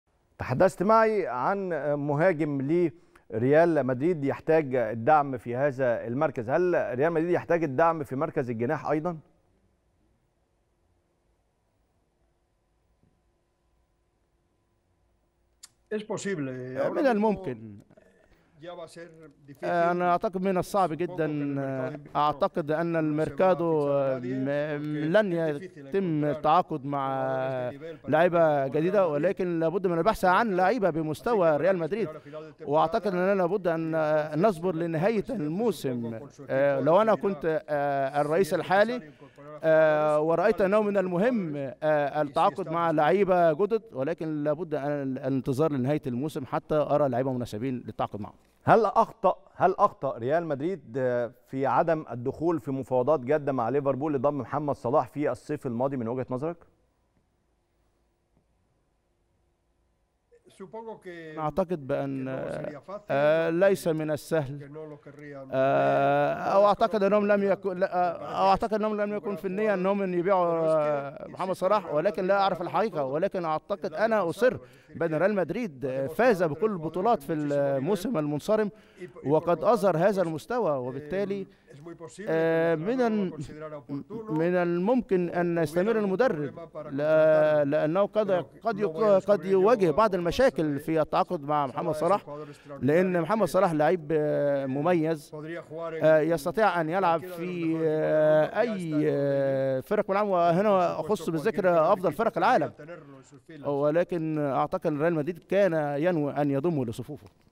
"محمد صلاح افضل لاعبي العالم-" 🔥🔥🔥رامون كالديرون: لو كنت رئيس نادي ريال مدريد حاليا كنت سضمه للفريق
"محمد صلاح افضل لاعبي العالم" 🔥🔥🔥رامون كالديرون رئيس نادي ريال مدريد الأسبق: لو كنت رئيس نادي ريال مدريد حاليا كنت سضمه للفريق